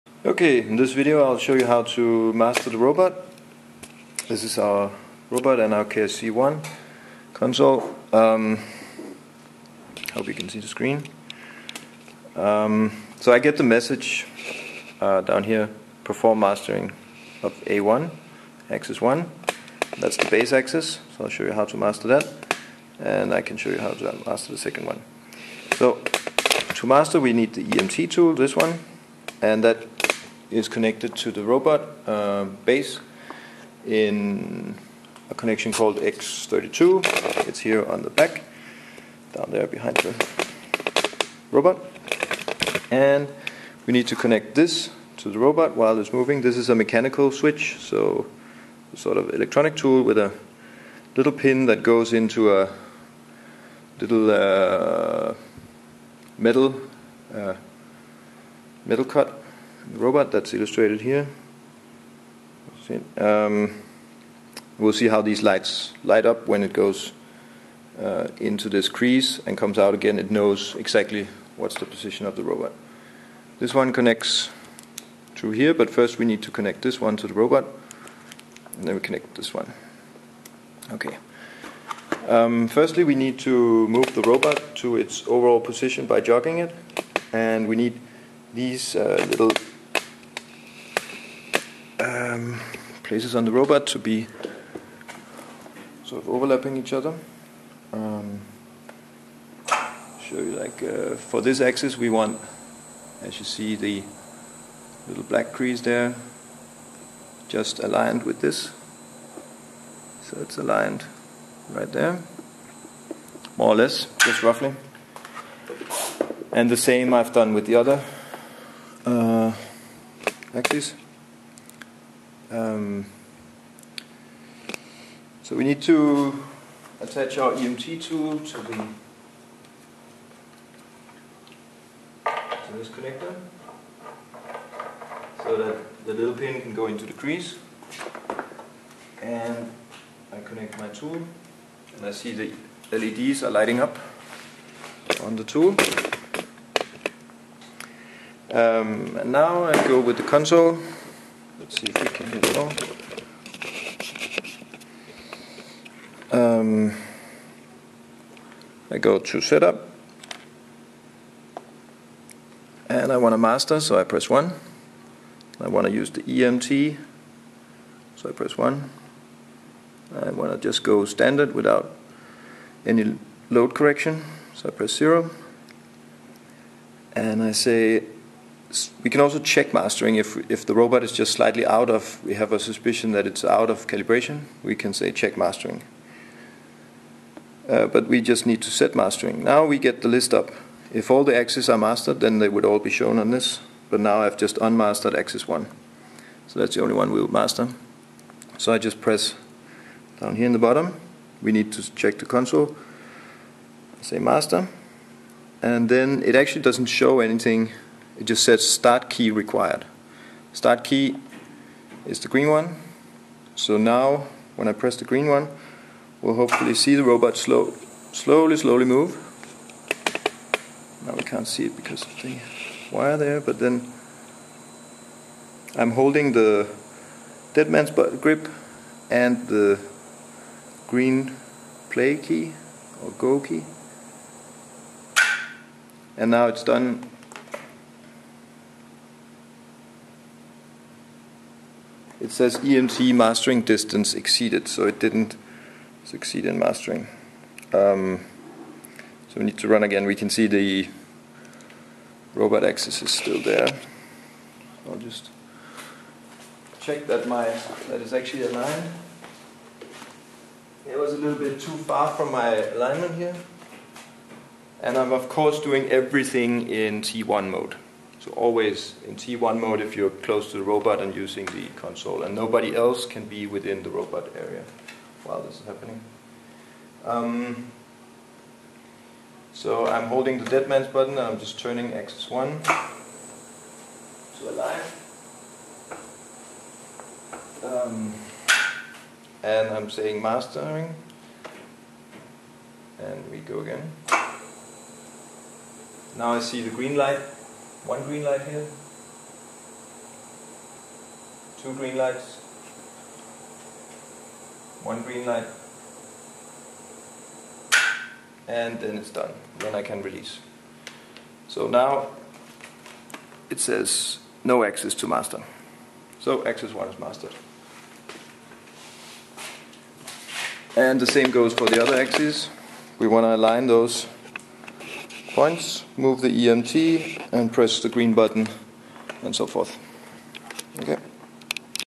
Mastering KR150 robot with EMT tool on KRC1
How to master KUKA robot using EMT and KRC1
MAKElab at Chalmers university of technology
Material Actuation and Kinetic Experimentation